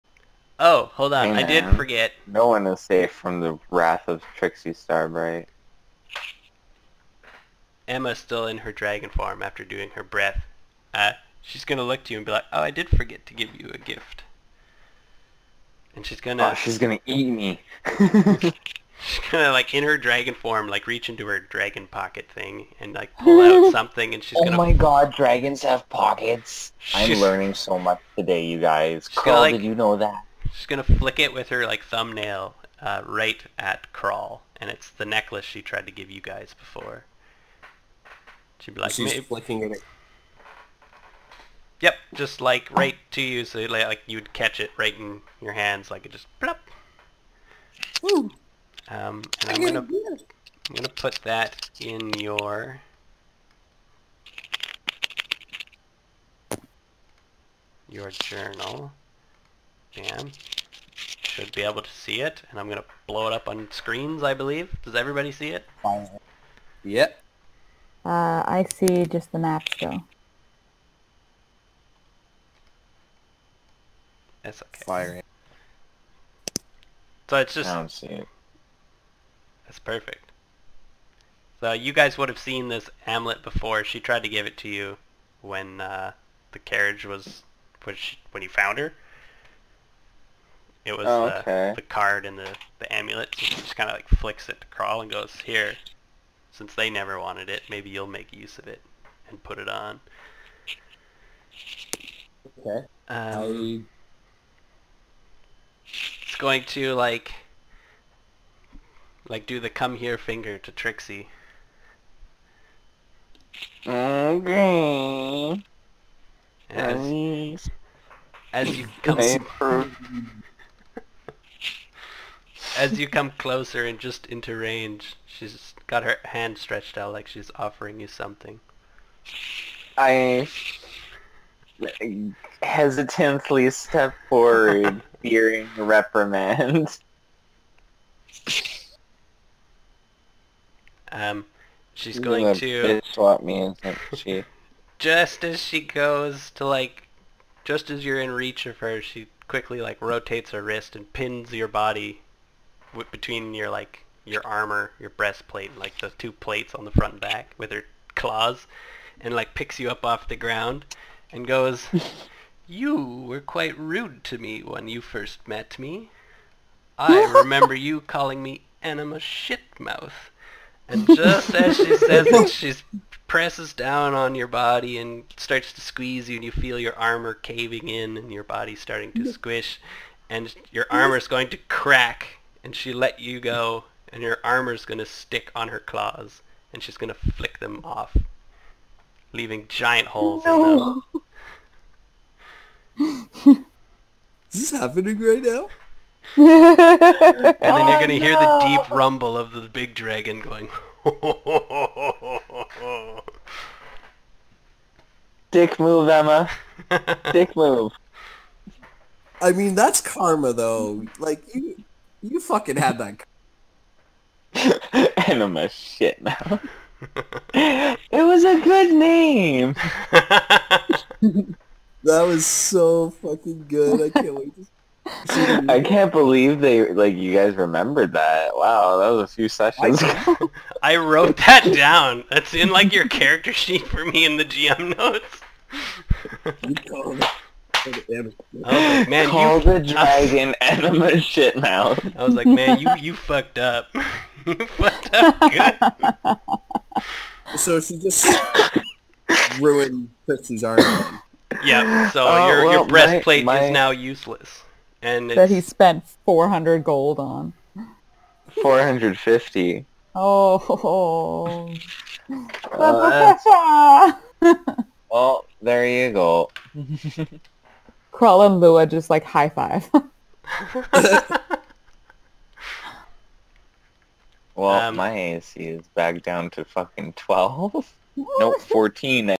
Avolars Gauntlet Emma destroys trixies armour
The party discovers that the little girl they "rescued" is a Dragon. she gives them tasks and some "gifts" and sets them on there way. Trixie did not know it but she made a grave mistake while mocking the little girls name.